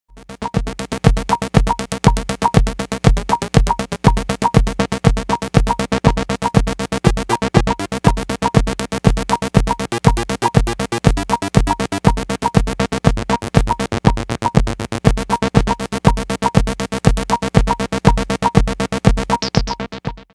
DSI Prophet '08 arpeggiator synced to Elektron MachineDrum
Here's the Prophet 08 arpeggiator synced to an Elektron MachineDrum beat with some really fake video footage from Mars.  ;-)